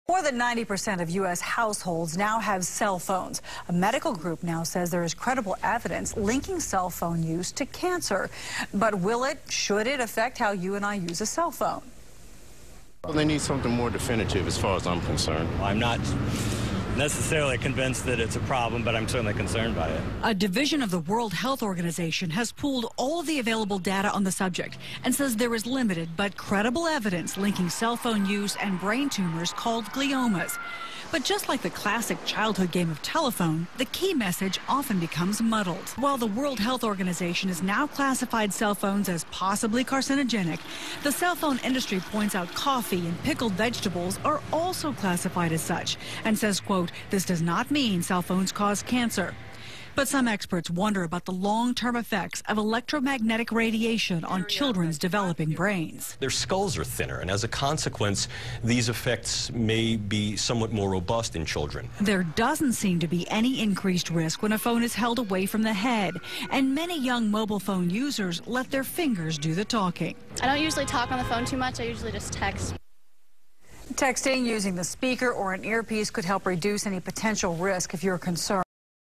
Cell phone cancer risk
A medical group now says there's credible evidence linking cell phone use to cancer.